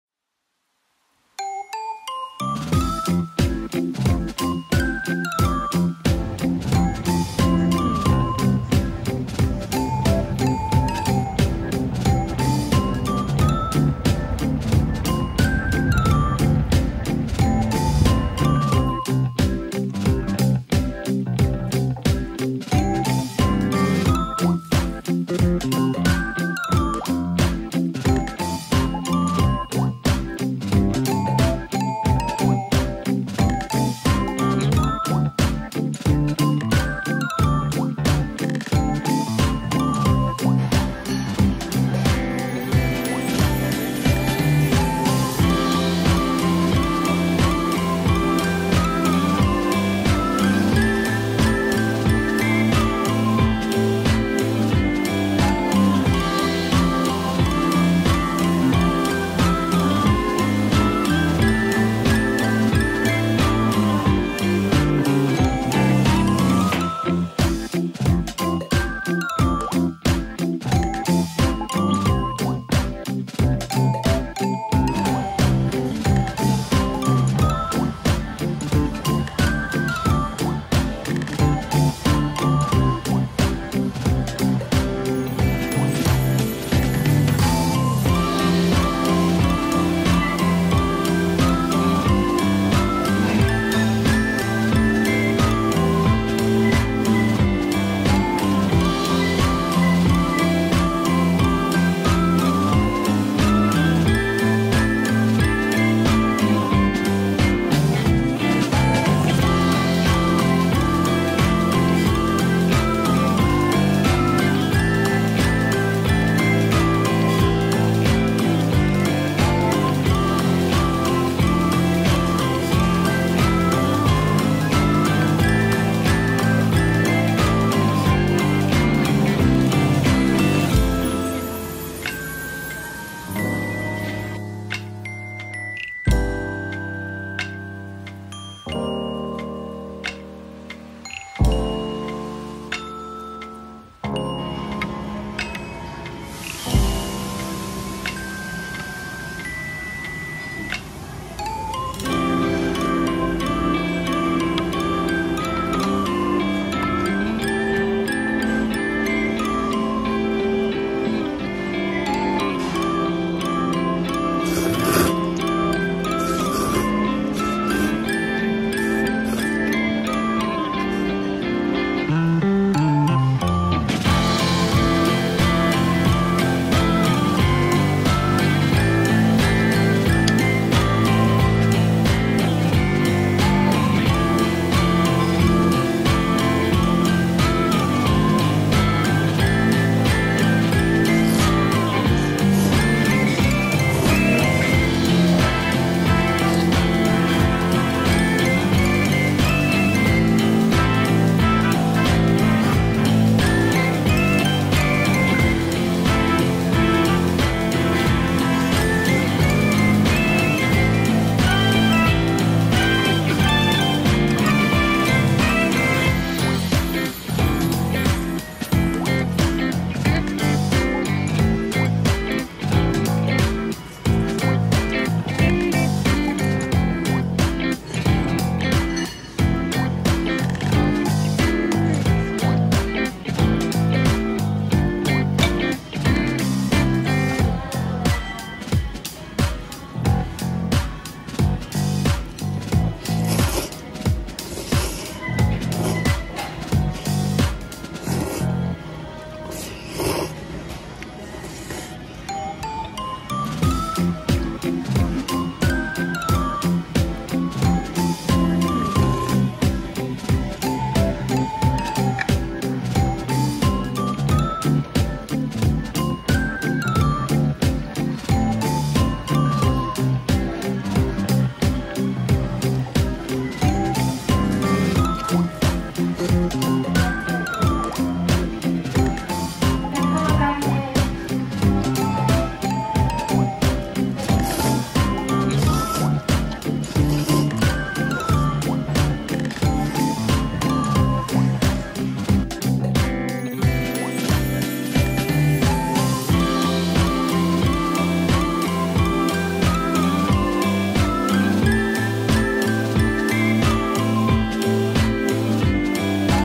【魁力屋】【京都北白川魁力屋】京都北白川ラーメン魁力屋岸里店〜特製醤油背脂ラーメン大＋唐揚げ定食＋餃子#魁力屋
夕方仕事終わり西成岸里の京都北白川ラーメン魁力屋で特製醤油背脂ラーメン大🍜＋唐揚げ定食＋餃子🥟京都背脂ラーメンコクがあってさっぱり細麺美味しい〜唐揚げと餃子🥟も美味しい〜満腹でした。
京都北白川ラーメン魁力屋ホームページ